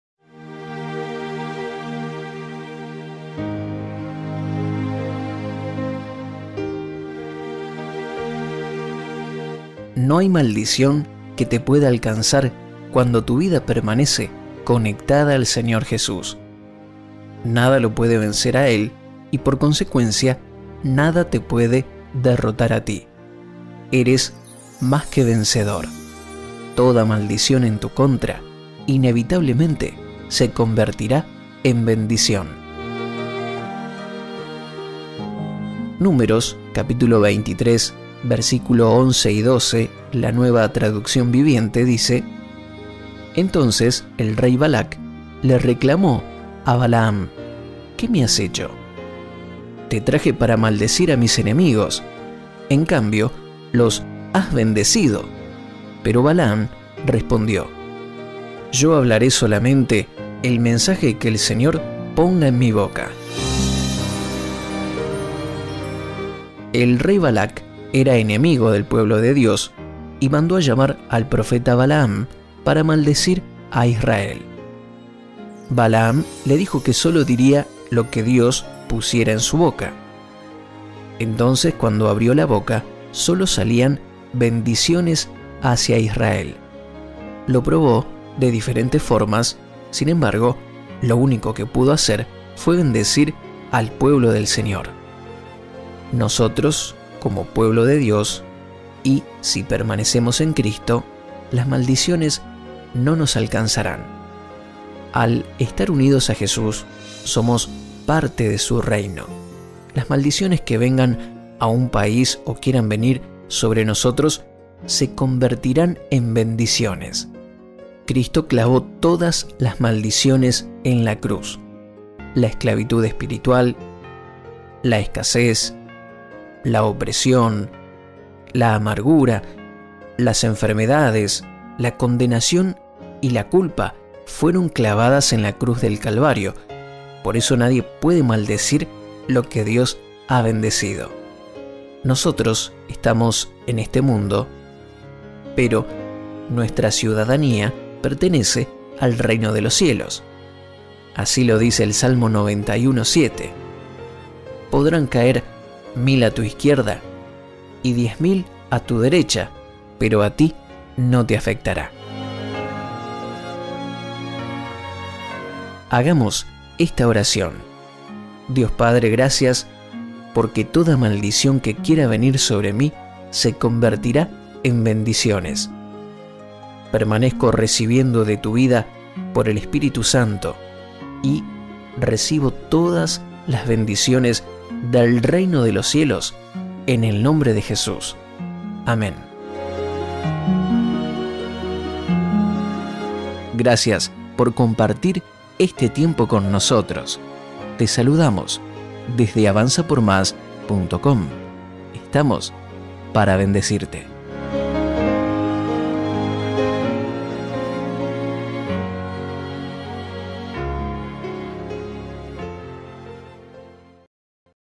148 - NADA PUEDE MALDECIR Lo Que Dios ha Bendecido
"No hay maldición que te pueda alcanzar cuando tu vida permanece conectada al Señor Jesús. Nada lo puede vencer a Él, y por consecuencia, nada te puede derrotar a ti. Eres más que vencedor. Toda maldición en tu contra, inevitablemente, se convertirá en bendición".